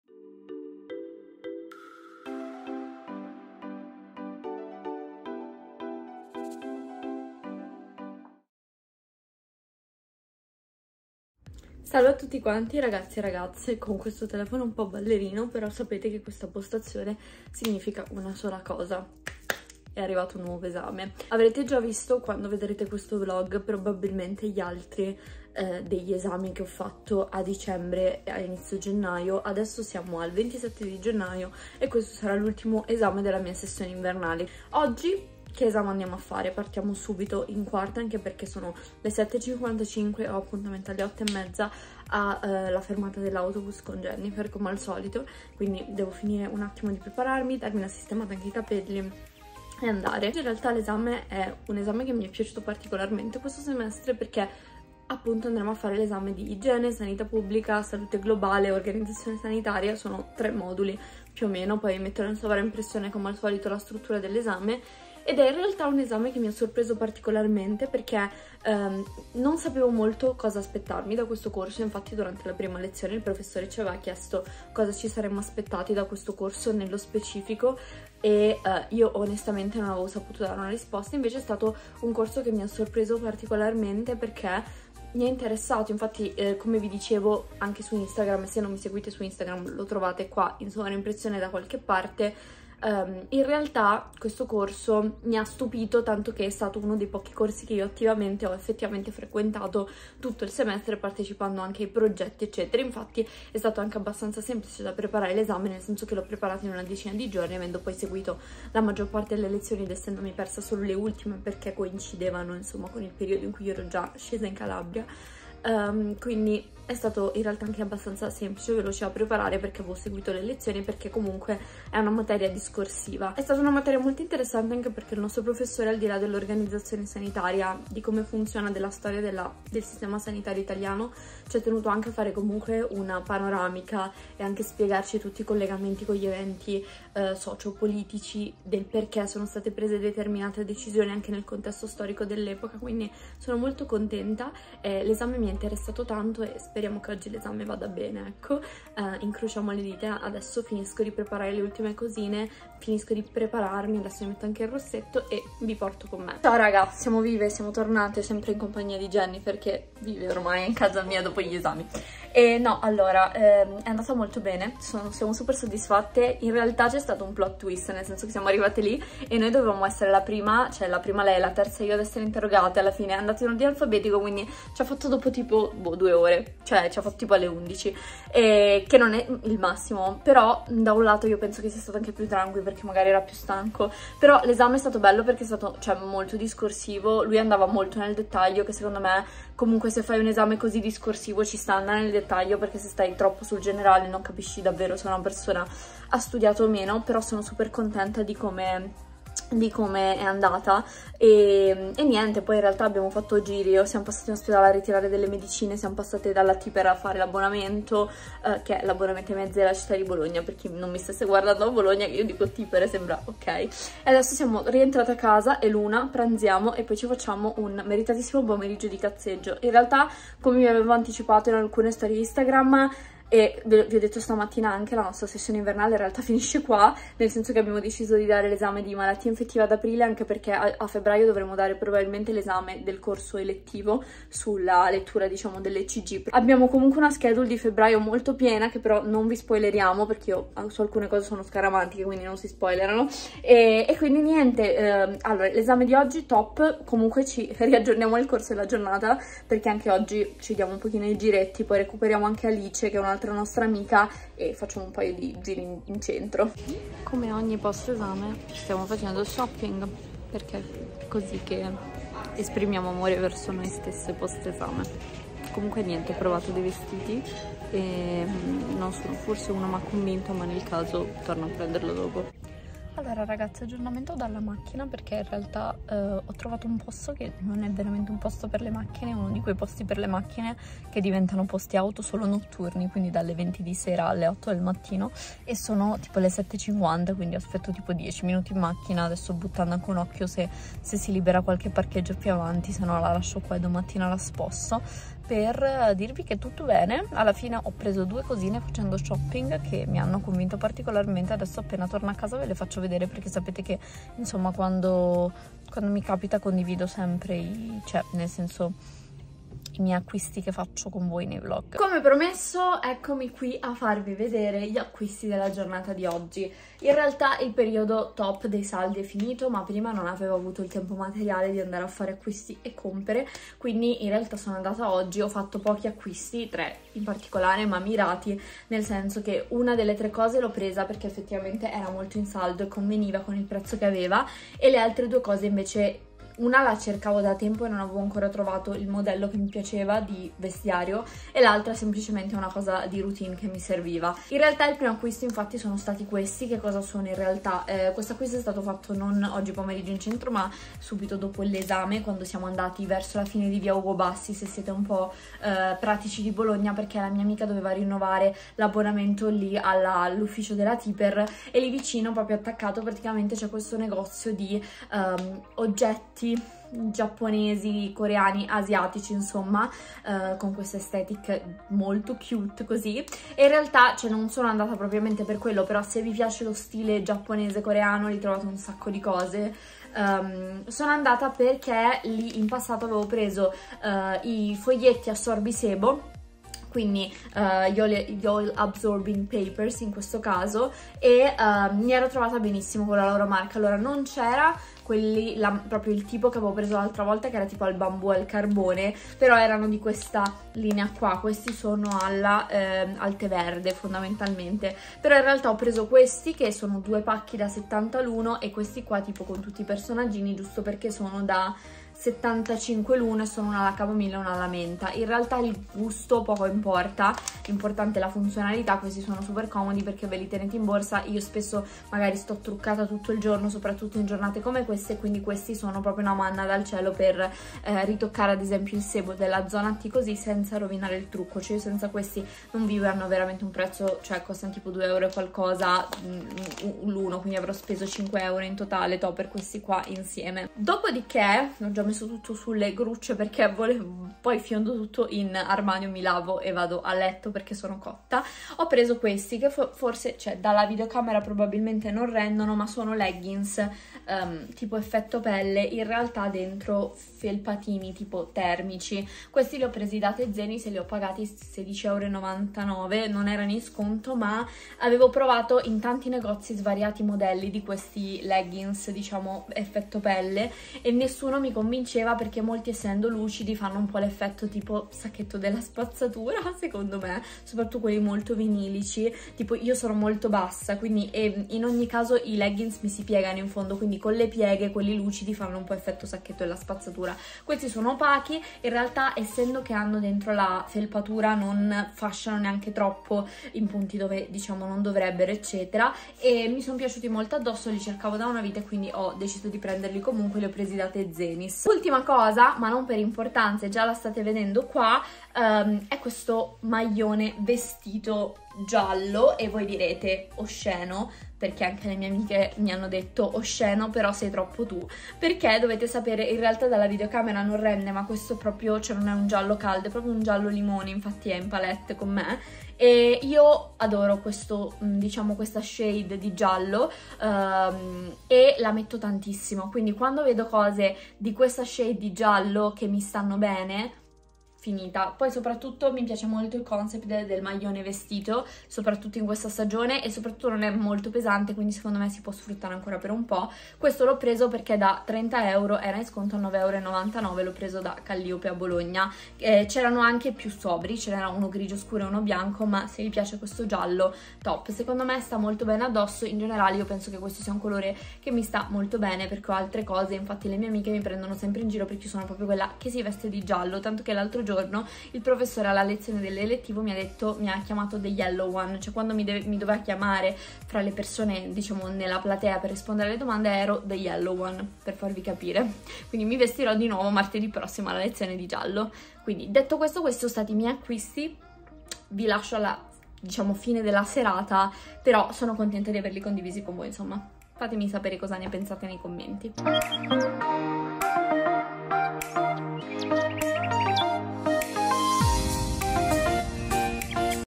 PRIMO ESAME DELLA SESSIONE INVERNALE! ❄️ || University Vlog #55 📚
Procediamo con questa lunga serie di Vlog dell'anno 2023! In questo vlog, cominciava la mia sessione invernale, con un esame che mi è piaciuto molto: Igiene e Salute Pubblica! Secondo me è stato gestito davvero bene ed è stato un esame che mi ha lasciato davvero tante conoscenze in più!